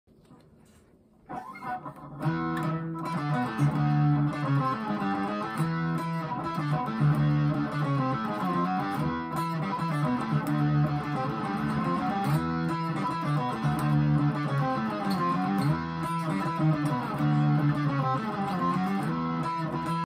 Boss OC-5 Octave Pedal
A new riff I wrote messing around with a Boss OC-5 octave pedal I just picked up.